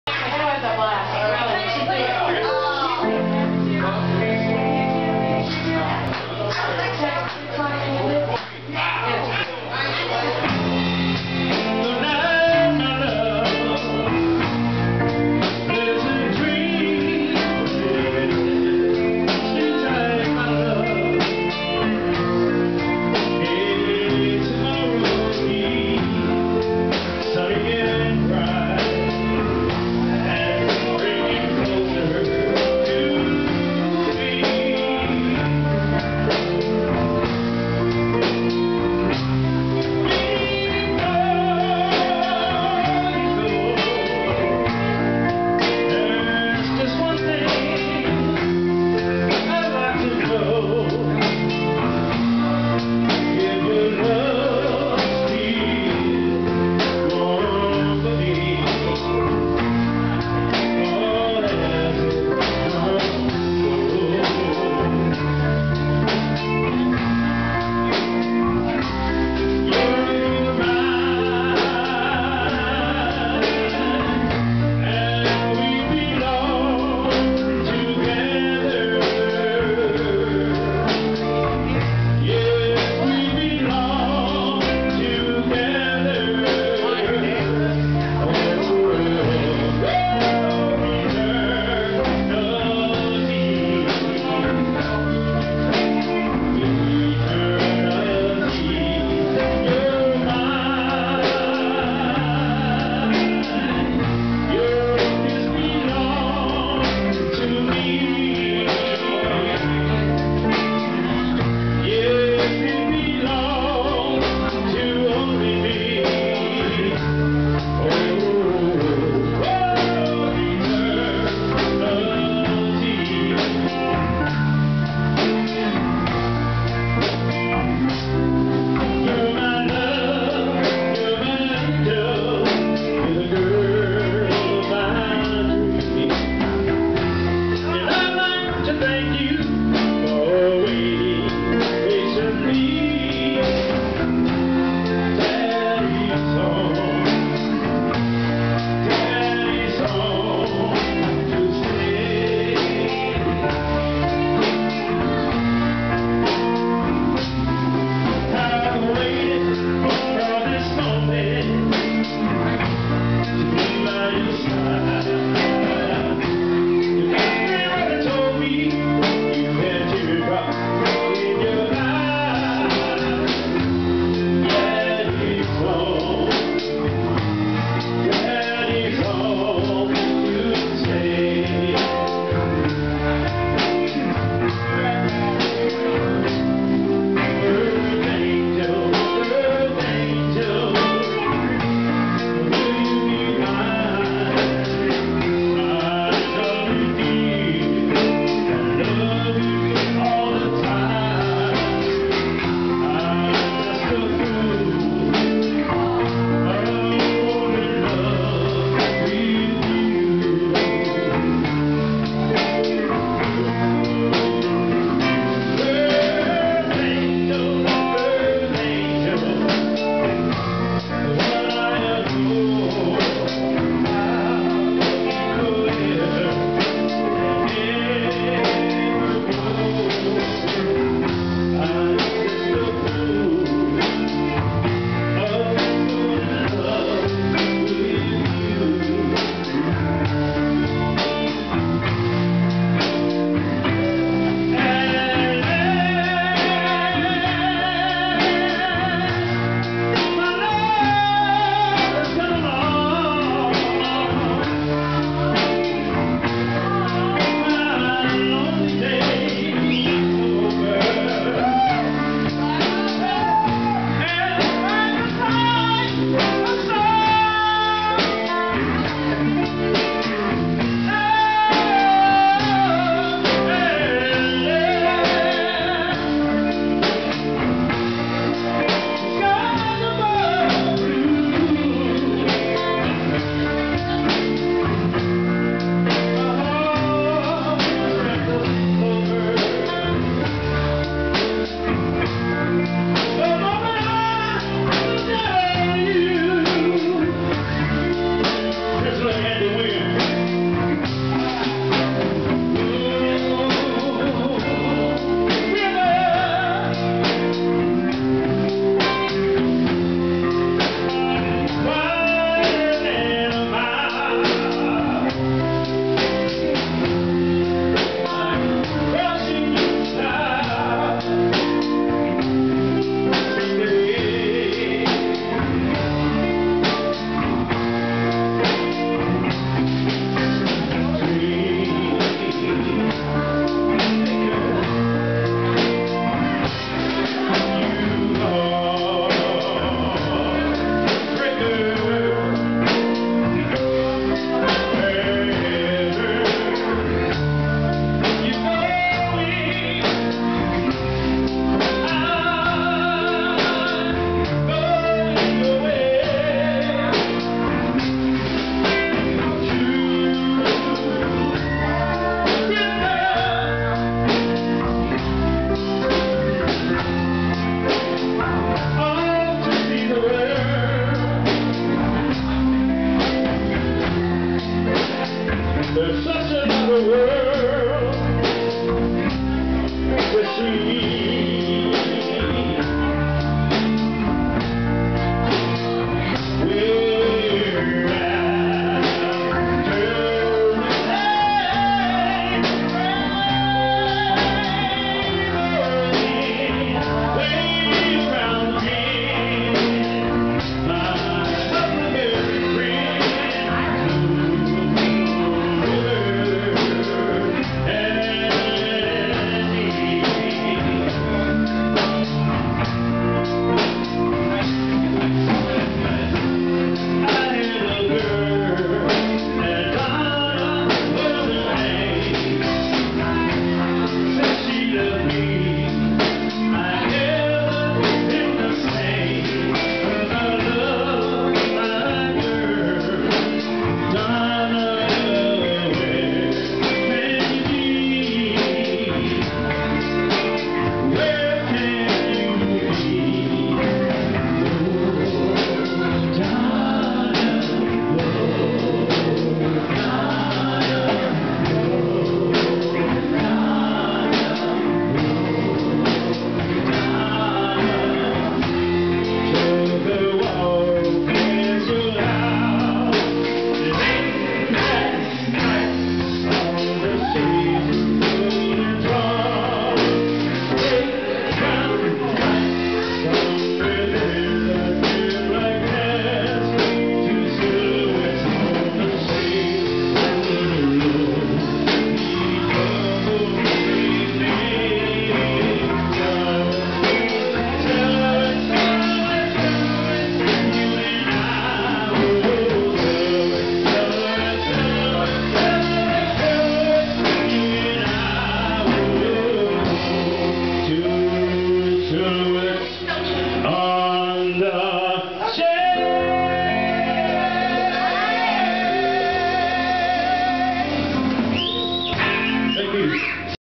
WEIRD HAROLD AND THE RUBBERBAND WITH JAMES TARVER 2/13/10 50's Medley
This was at the Mena Eagles club Vanentines dance 2010 James and Lisa Tarver sitting in with the Rubberband we all had a Blast!